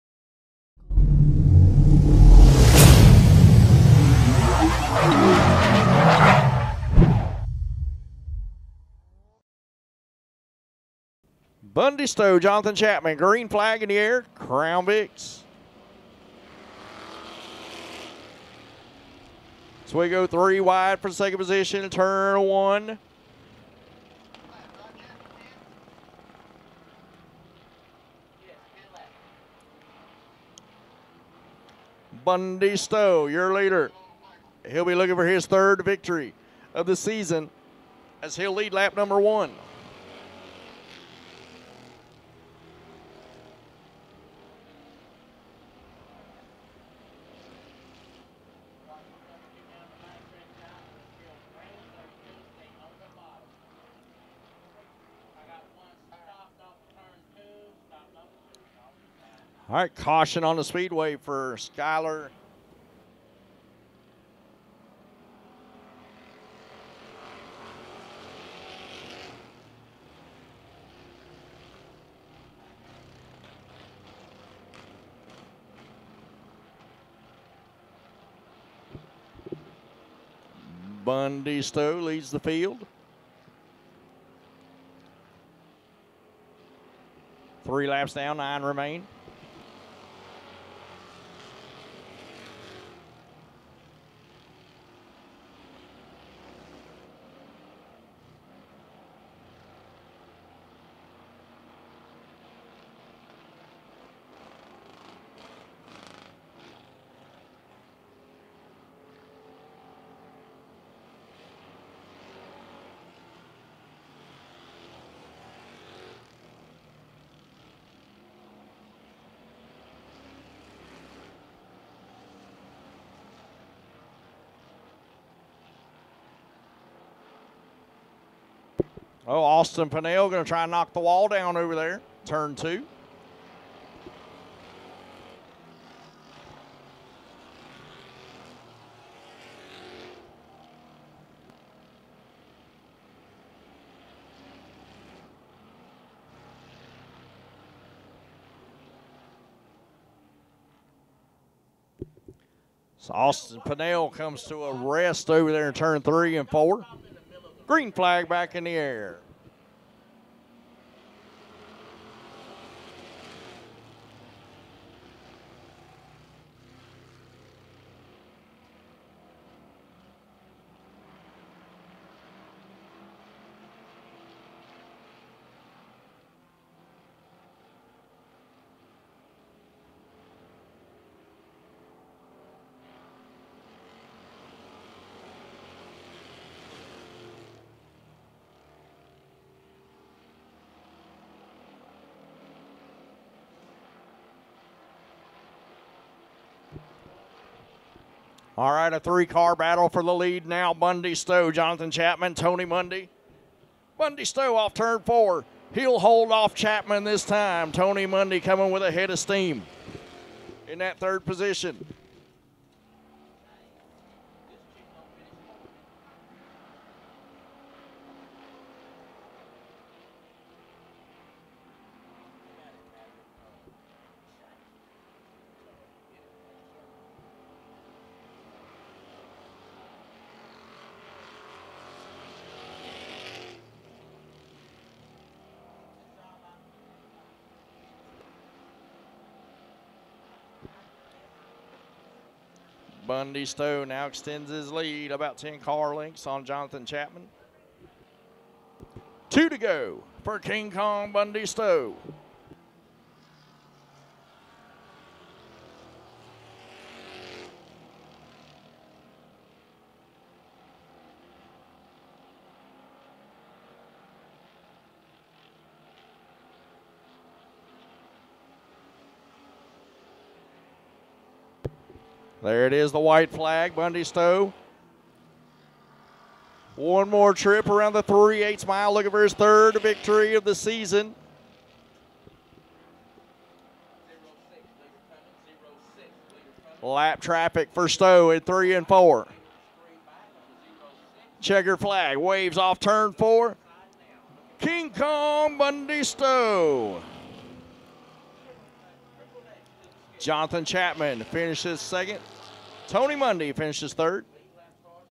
Crown Vic Feature - Carolina Speedway 5/10/24
Full race (10 laps)

A huge thank you to JP Performance for sponsoring this weekend's videos!

Looking to buy some BAM Racing Videos merchandise?